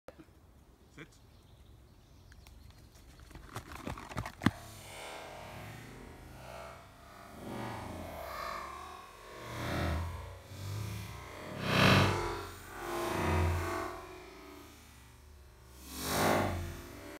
Black puppy jump fail
Slow motion video of Winny the Labrador/Visla trying to grab a stick.